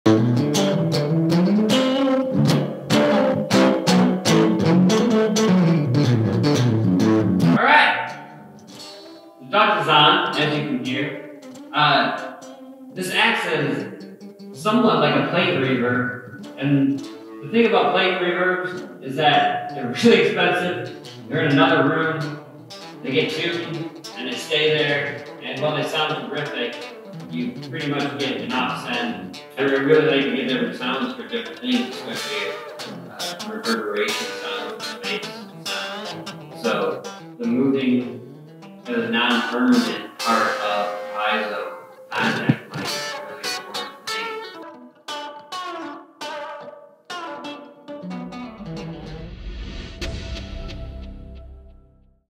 Jordan Pries explains his Homemade Duct-Verb and Jams Out
Jam sesh with Jordan Pries (The Arc of All) and some insight on the homemade duct-verb. Be sure to check out BlackCircleRadio and SourceOfLightAndPower